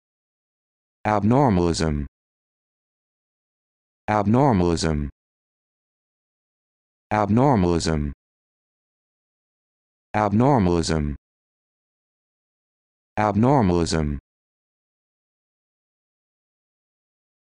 How to Pronounce abnormalism
This video shows you how to pronounce abnormalism